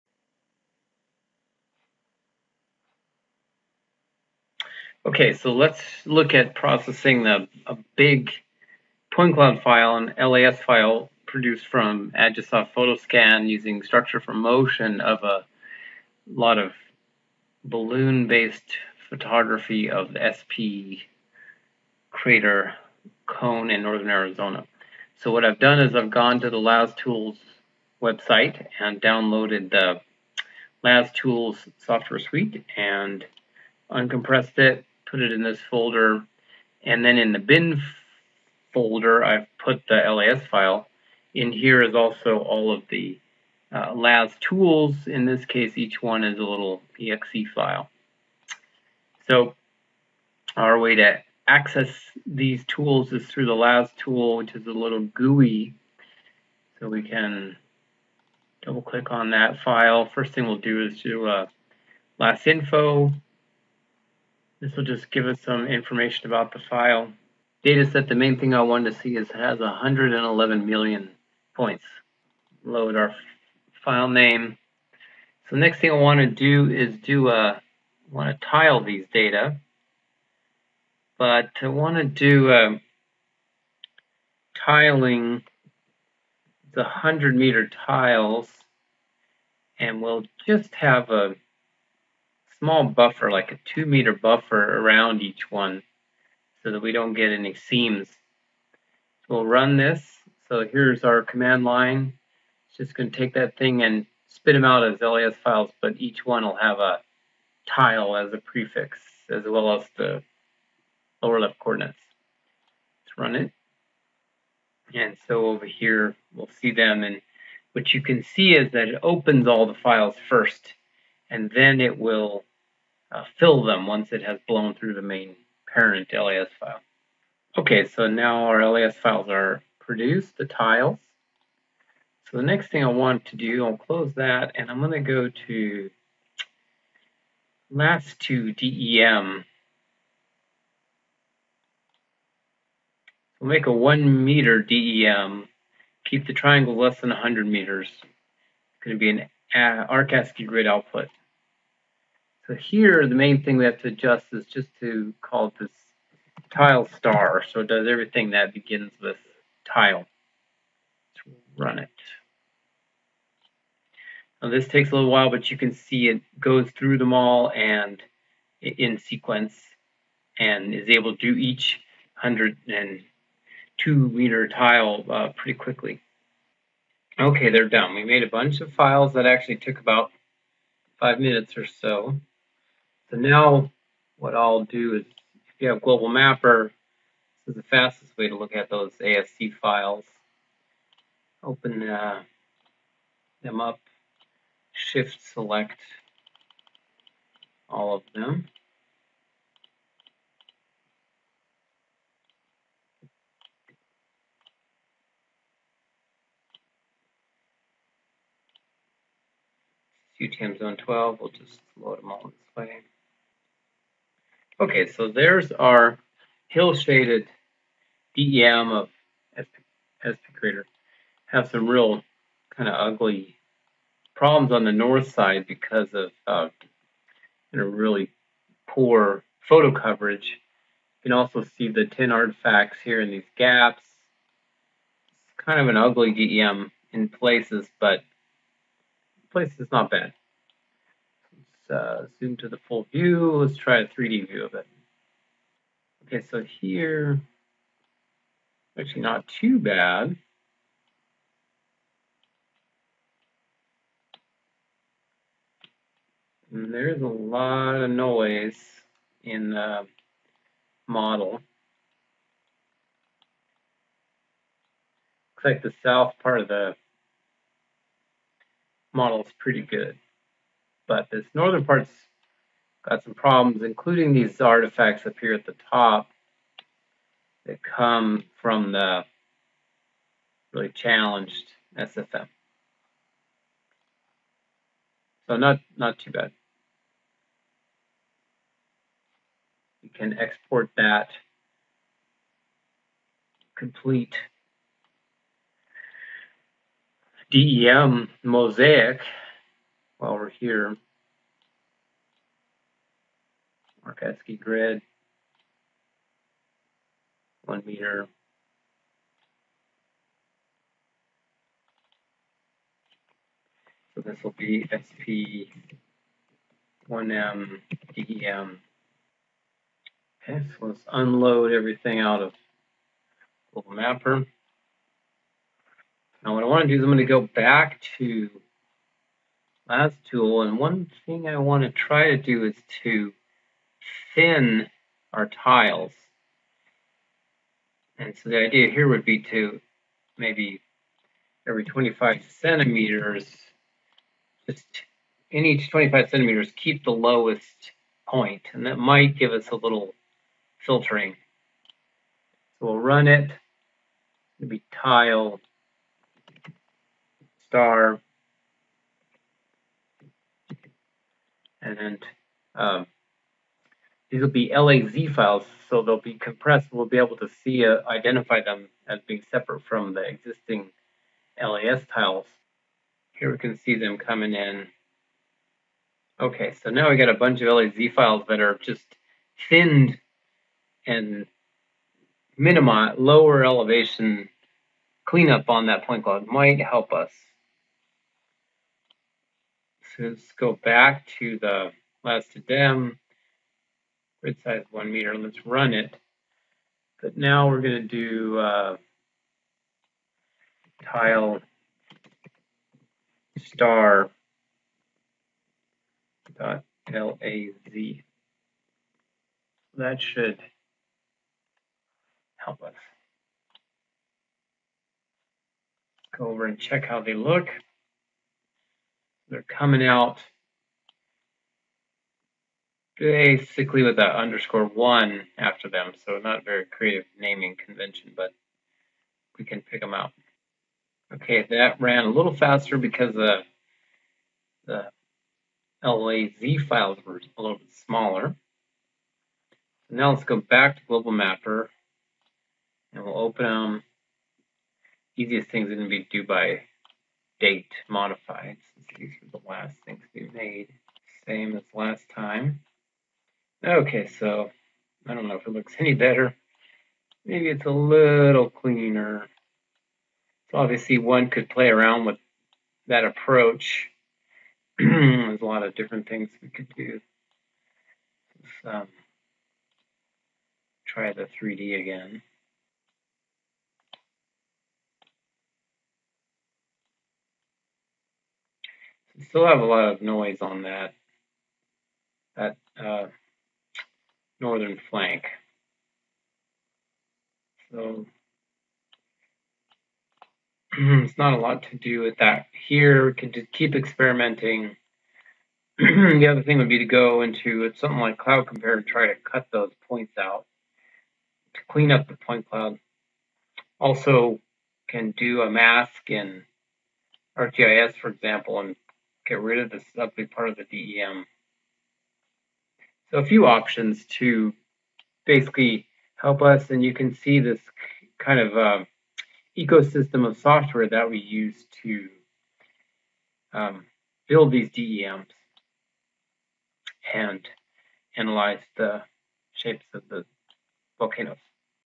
Processing a big SfM point cloud with Lastools
Processing a 111M point point cloud produced using Agisoft Photoscan Structure from Motion from SP Crater Arizona. I show how to use the lasinfo, lastile, lasdem, and lasthin commands to explore the DEMs and also visualize the resulting DEMs using GlobalMapper.